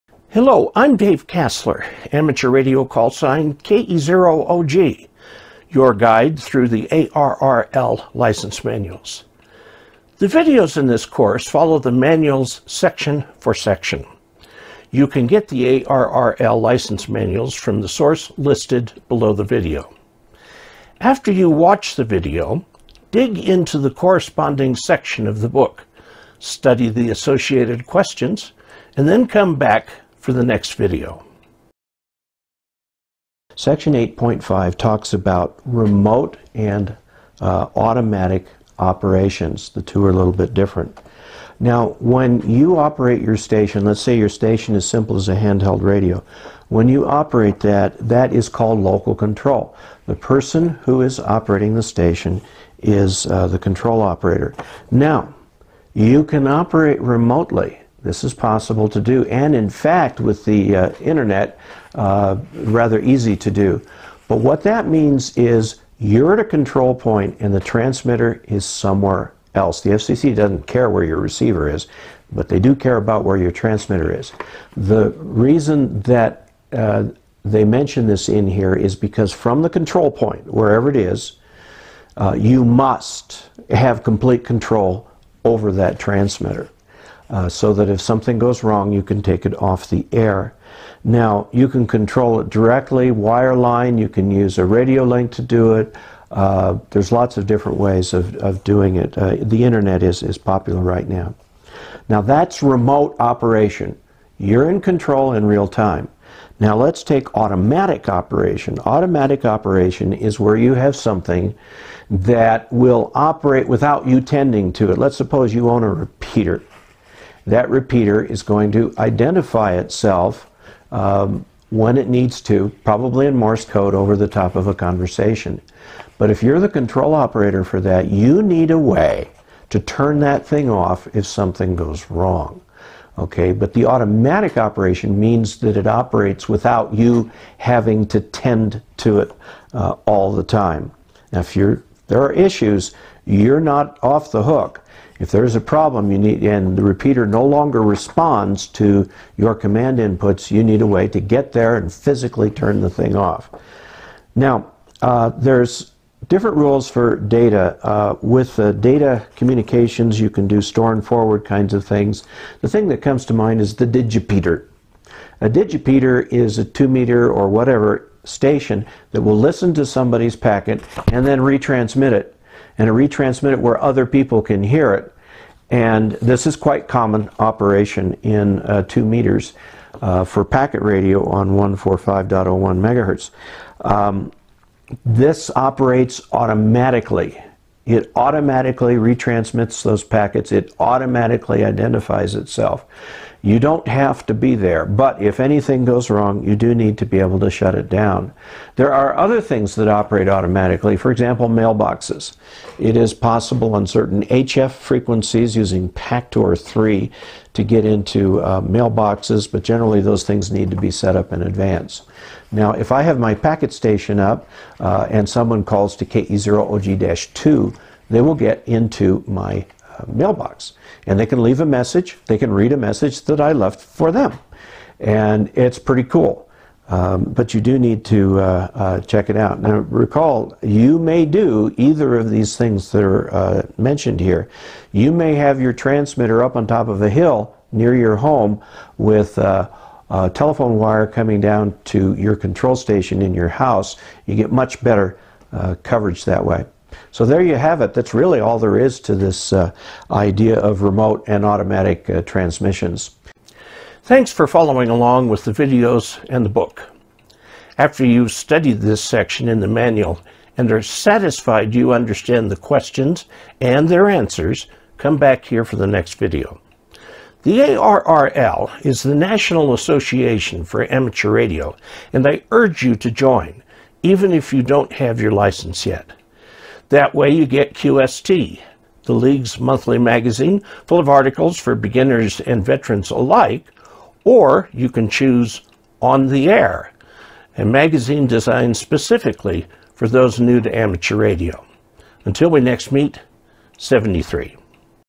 Dave Casler Technician License Series: T30 Section 8.5 Remote and Automatic Operation 2022-2026
Do you have to be at the control point of your radio when operating? This video explains section 8.5, Remote and Automatic Operation to help you understand the requirements of remote operation.

Follow along with Dave as he takes you through the ARRL Ham Radio License Manual: